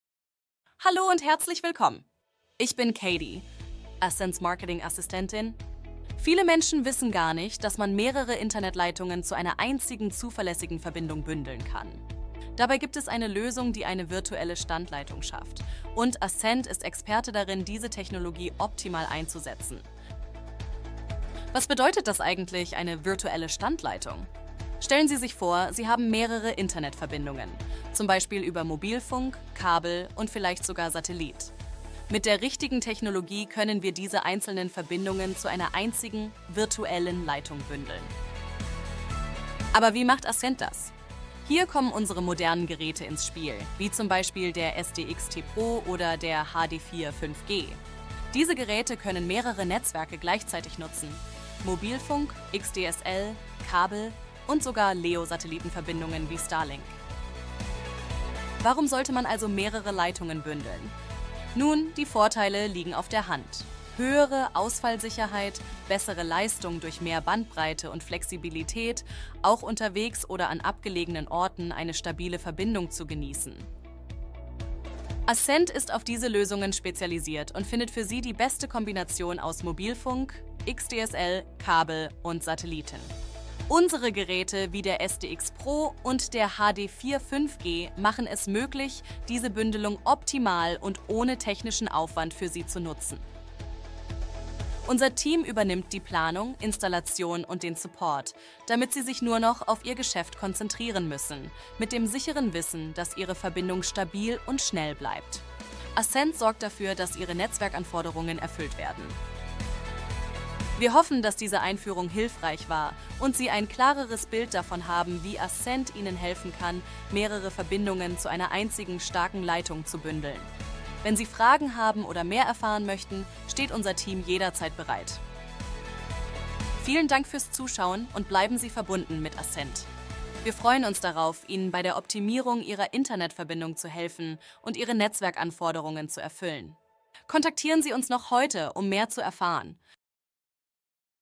Virtuelle Standleitung – Ascend GmbH mit 5G, xDSL & Starlink gebündelt in einer virtuellen Leitung
In diesem 1-minütigen Video erfahren Sie, wie Sie mit virtuellen Standleitungen und der Bündelung mehrerer Internetverbindungen, wie Mobilfunk, LEO-Satelliten (z. B. Starlink), xDSL und Kabel, eine stabile und zuverlässige Internetverbindung schaffen können. Wir zeigen, wie Unternehmen mithilfe moderner Router und Technologien eine ausfallsichere, schnelle Verbindung aufbauen können – ideal für konstante Konnektivität in jeder Situation. Entdecken Sie, wie die Hardware von ASCEND, darunter SDX Pro und HD4 5G, maximale Internetstabilität ermöglicht.

Mehr Informationen finden Sie auf unserer Website!